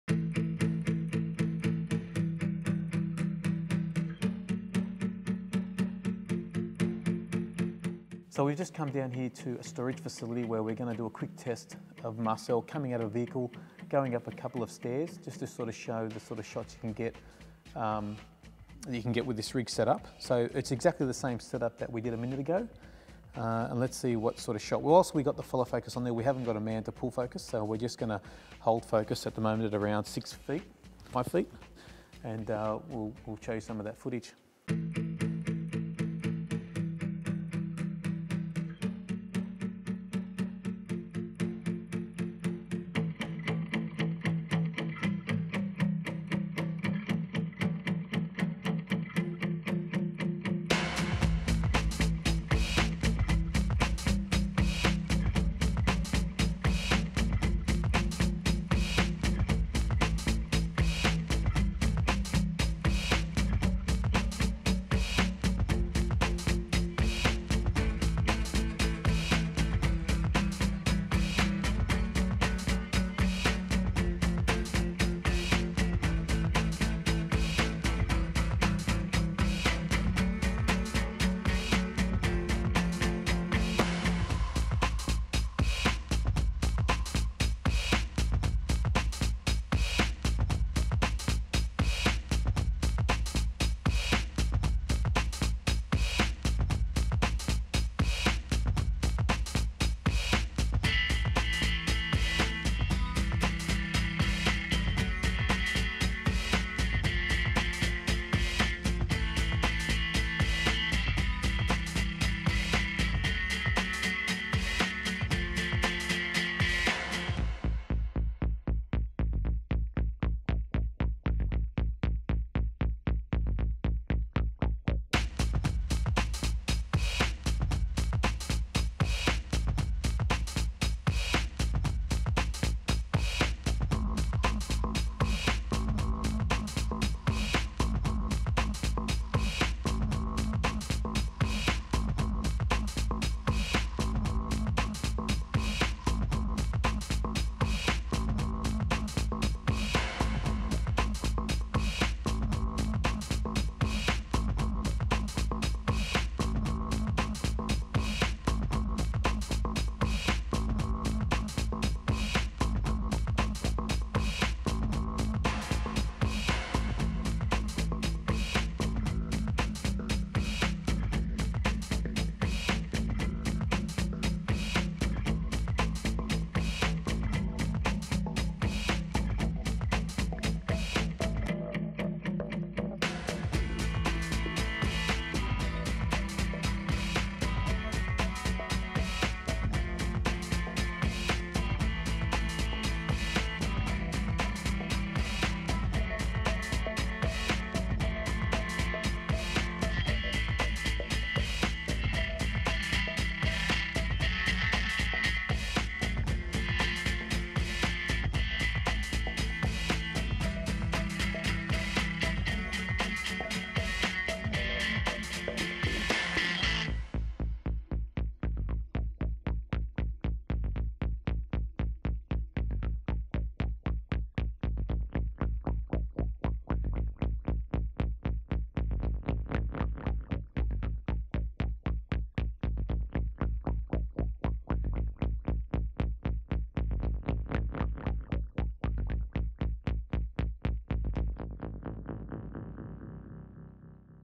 DJI Ronin Sample Footage
After completing the instructional video on how to balance the DJI Ronin we took the setup to storage facility to gather some sample footage. We took one take of each operational mode, Upright, Briefcase and Normal Mode.
Whilst the Redrockmicro wireless follow focus was installed and setup we did not operate the device during our shoot.